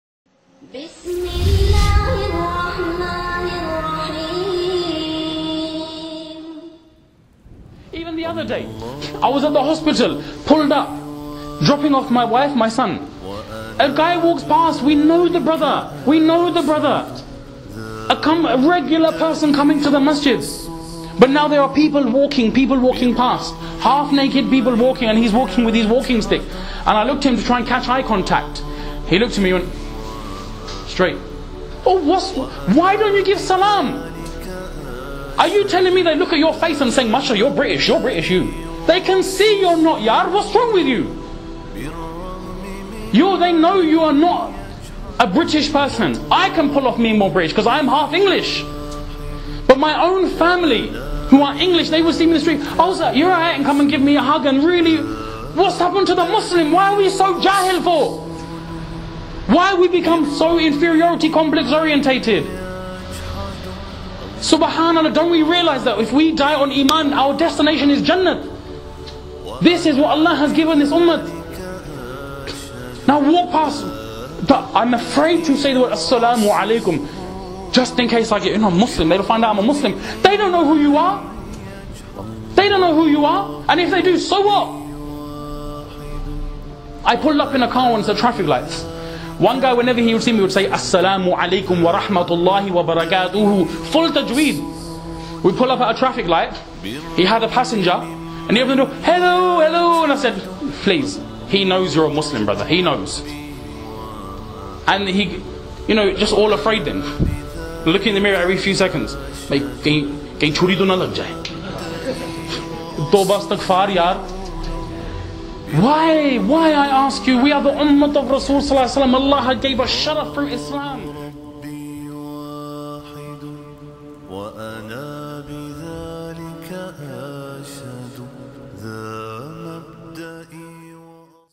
Too embarrassed to give salaam? Sheikh Abdul Majid
Too embarrassed to give salaam?
Sheikh Abdul Majid
This lecture has been subtitled to accommodate for our deaf brothers and sisters.

Sadly there are some Muslims that think Islam is something for indoors only, but they fail to understand Islam is a way of life, and a beneficial way of life which needs to be shown and shared with others. It's about time we became confident with our religion so we can propagate it as we should.
Shaykh talks about some of his unfortunate encounters with so called practicing Muslims. May Allah save us and making us such that he is pleased with us. Ameen.